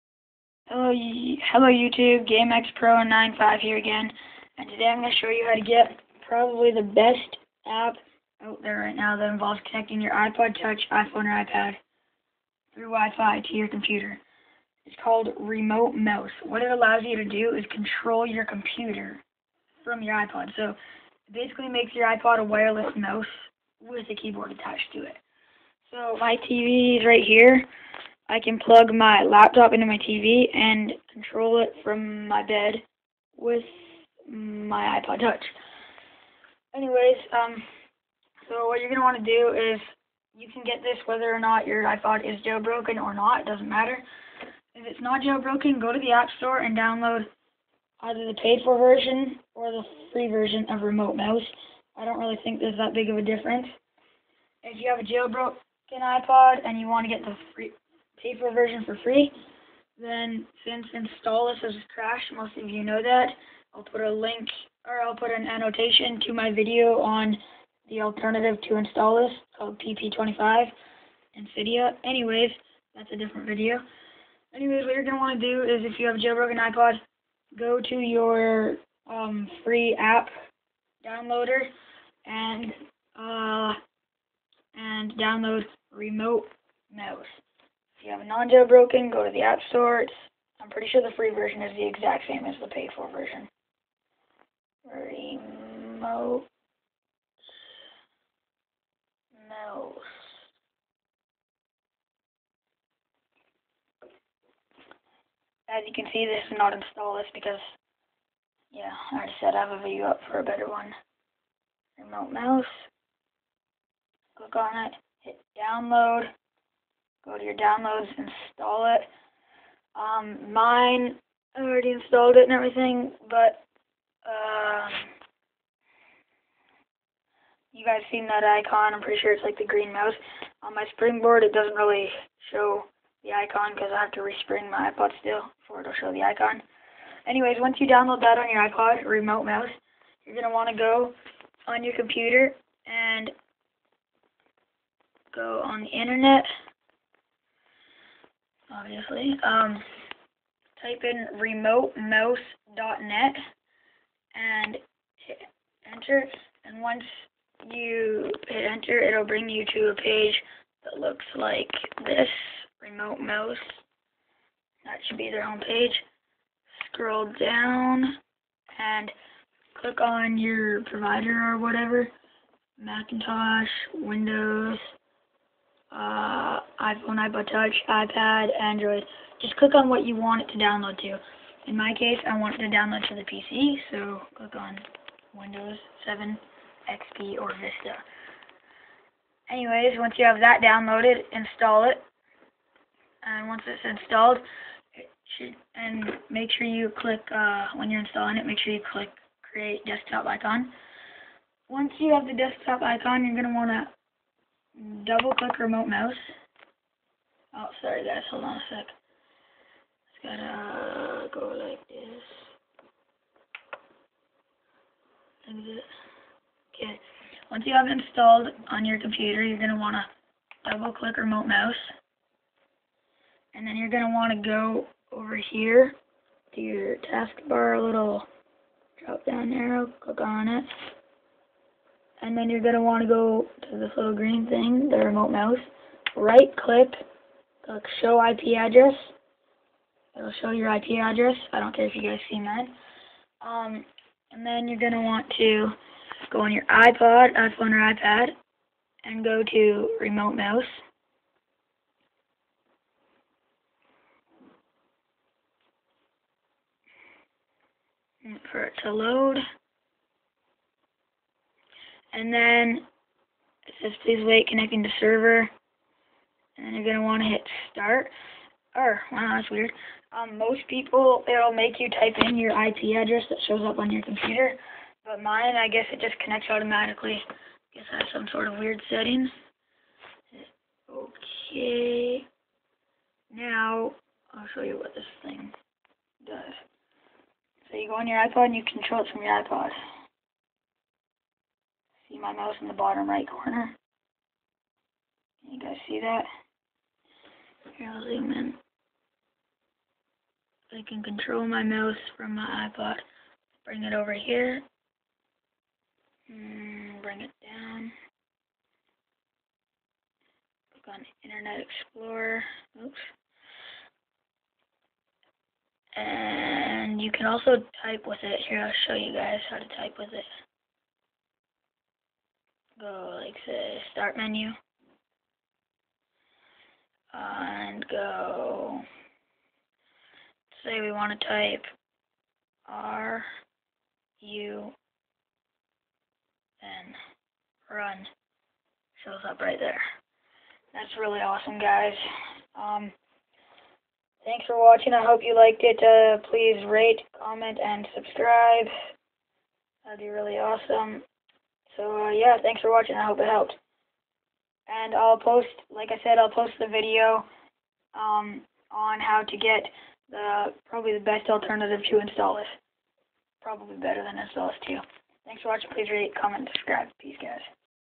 how to download install and use remote mouse
in this video i wil show you how to download install and use remote mouse which allows you to controll your pc from your ipod, iphone, or ipad